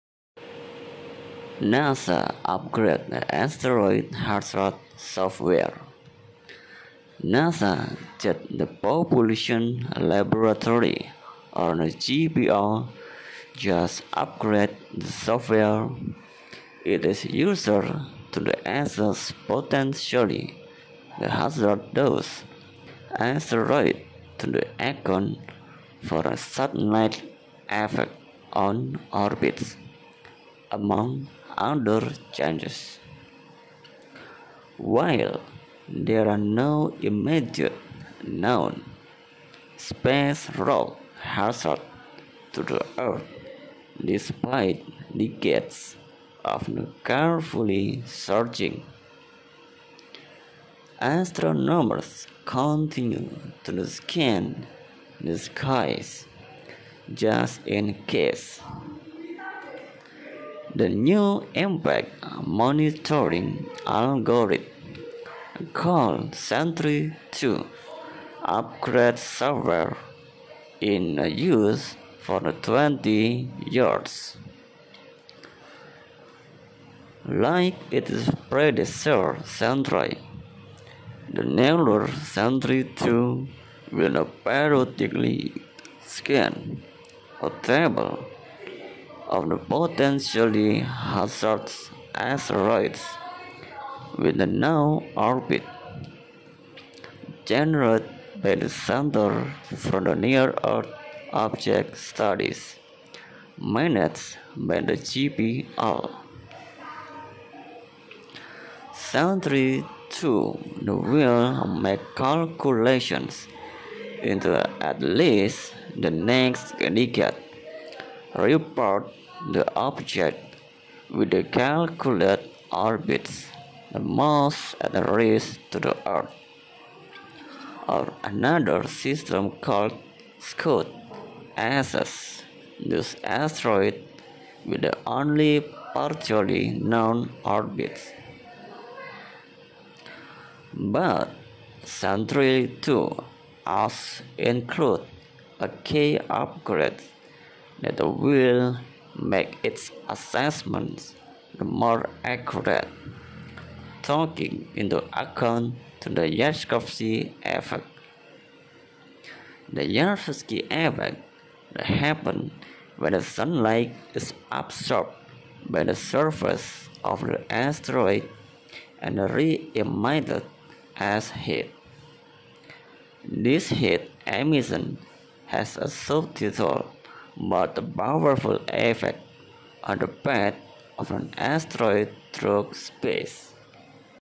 NASA Upgrades Asteroid Hazard Software
NASA's Jet Propulsion Laboratory (JPL) just upgraded the software it uses to assess potentially hazardous asteroids to account for sunlight's affect on orbits, among other changes. While there are no immediate known space rock hazards to Earth despite decades of careful searching, astronomers continue to scan the skies, just in case. The new impact monitoring algorithm, called Sentry-II, upgrades software in use for 20 years.

Like its predecessor Sentry, the newer Sentry-II will periodically scan a table of potentially hazardous asteroids with known orbits, generated by the Center for Near Earth Object Studies managed by JPL. Sentry-II will make calculations into at least the next decade, reporting the objects with calculated orbits most at risk to Earth. (Another system called Scout assesses those asteroids with only partially known orbits.)
But Sentry-II also includes a key upgrade that will make its assessments more accurate: taking into account the Yarkovsky effect.
The Yarkovsky effect happens when sunlight is absorbed by the surface of the asteroid and re-emitted as heat. This heat emission has a subtle, but powerful effect on the path of an asteroid through space